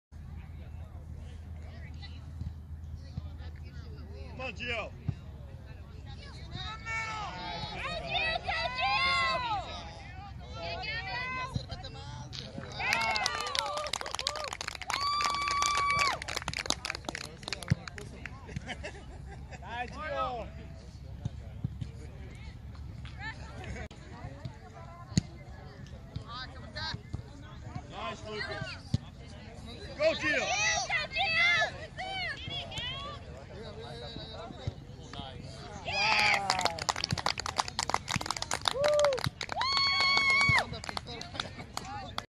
10-20-18 Gio's goals B05 Titans Navy Puyallup vs EW Surf Win 10-0
Sat Oct 20, 2018 Plantes Ferry Park Spokane WA